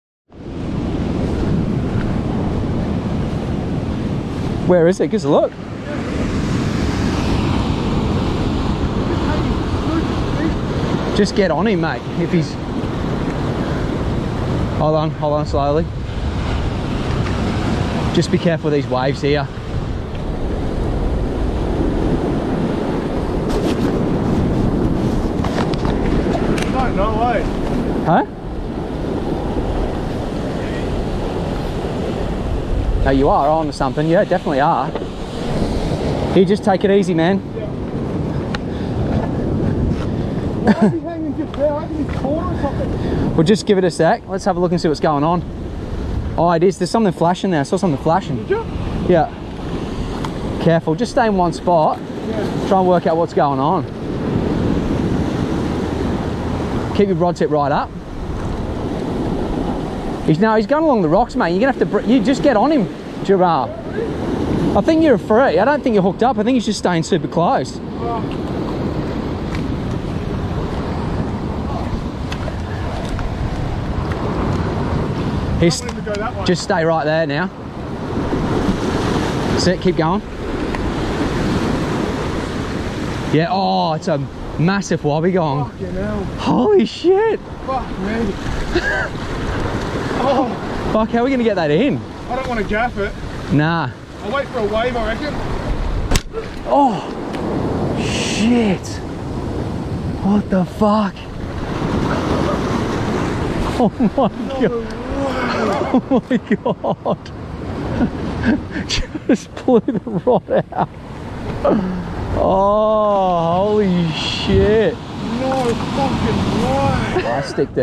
TRAGEDY ON THE ROCKS - R.I.P. PENN PREVAIL - ROCK FISHING TWEED HEADS
Short clip of a recent outing to Fingal Heads chasing the elusive East